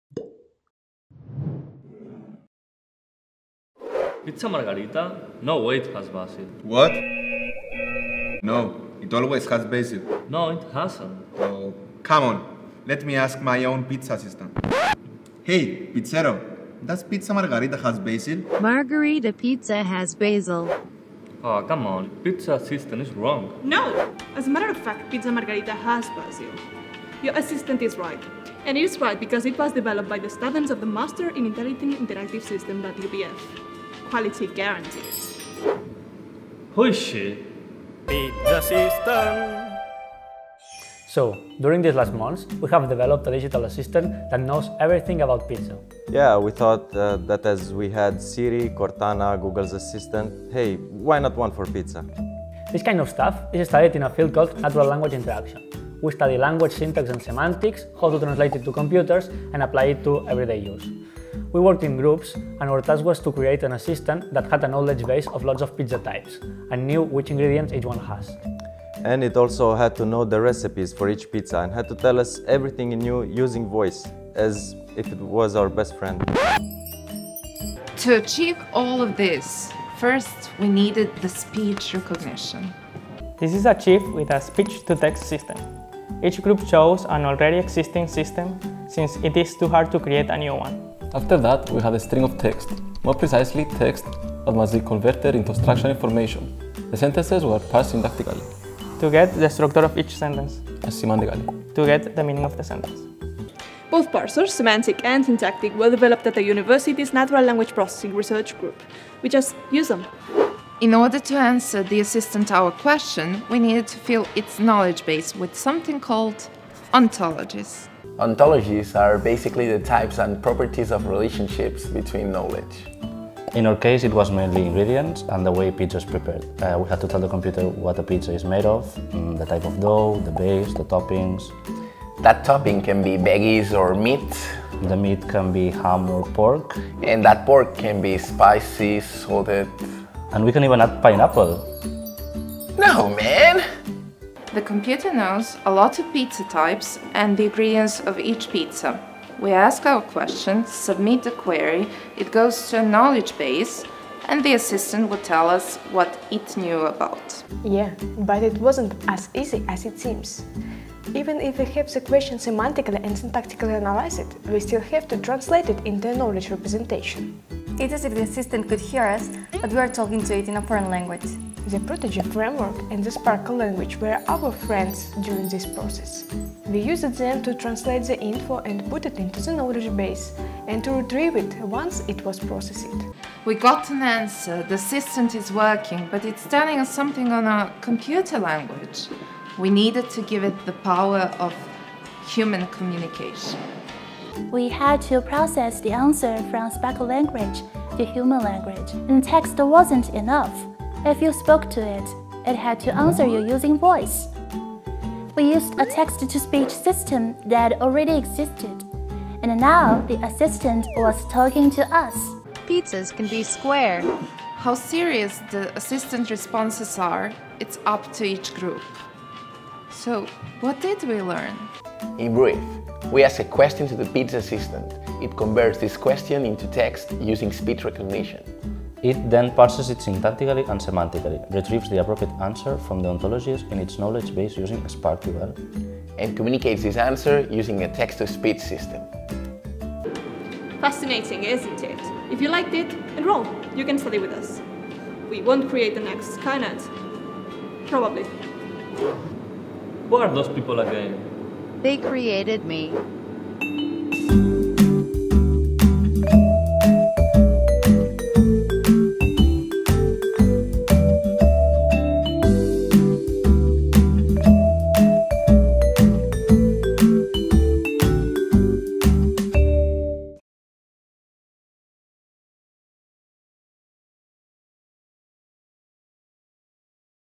HOW TO INTERACT WITH A COMPUTER PIZZA ASSISTANT?
This video shows what can we do, what do we learn, and how do we enjoy dealing with communicating with computers in the Natural Language Interaction course from the Master in Intelligent Interactive Systems at the Universitat Pompeu Fabra. A computer-based pizza assistant is created by using speech recognition, semantic and syntactic parsing, ontologies, and speech synthesis.
What are the ingredients of a Pizza Margarita? Try it!